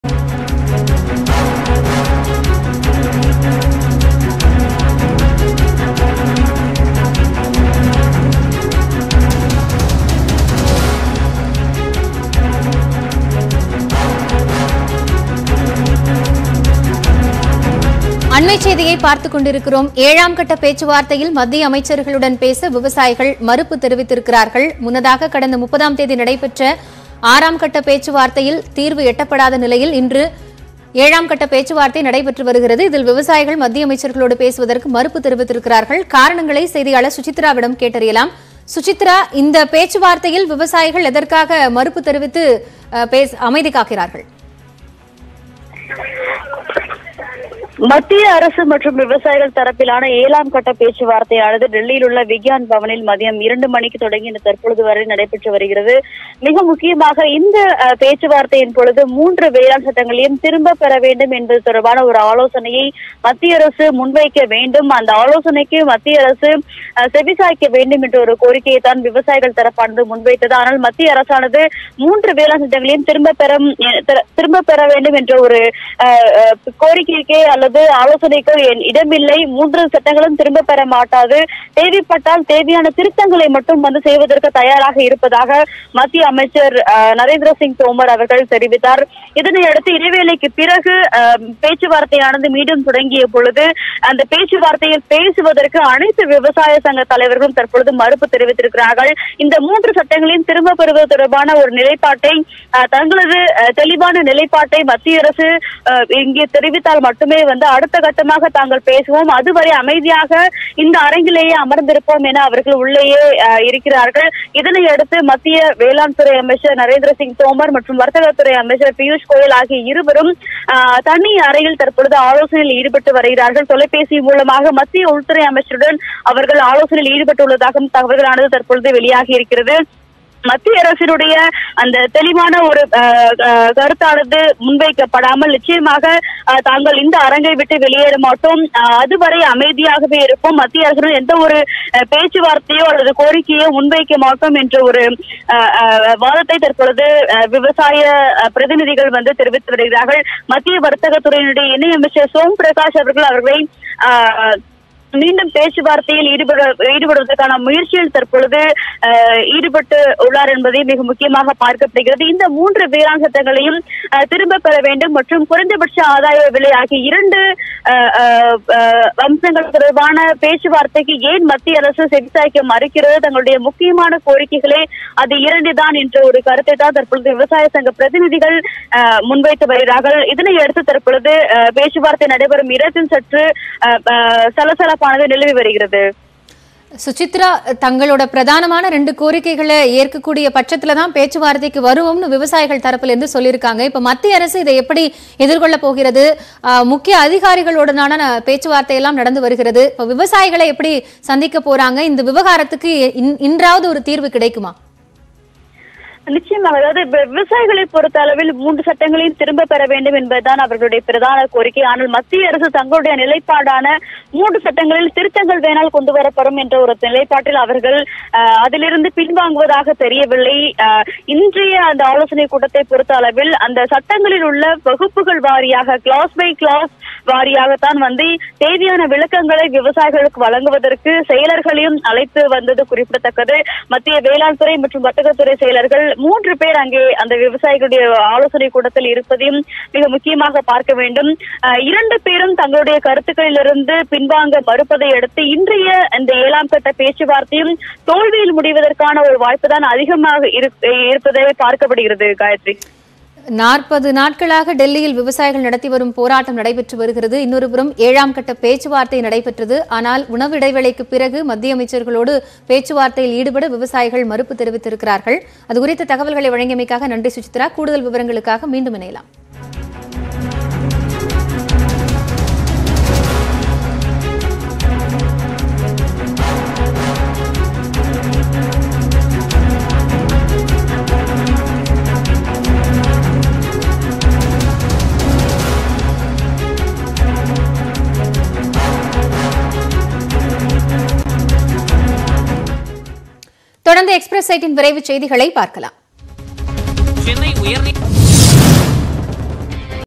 Breaking News | 7-வது கட்ட பேச்சுவார்த்தையில் மத்திய அமைச்சர்களுடன் பேச விவசாயிகள் மறுப்பு
7-வது கட்ட பேச்சுவார்த்தையில் மத்திய அமைச்சர்களுடன் பேச விவசாயிகள் மறுப்பு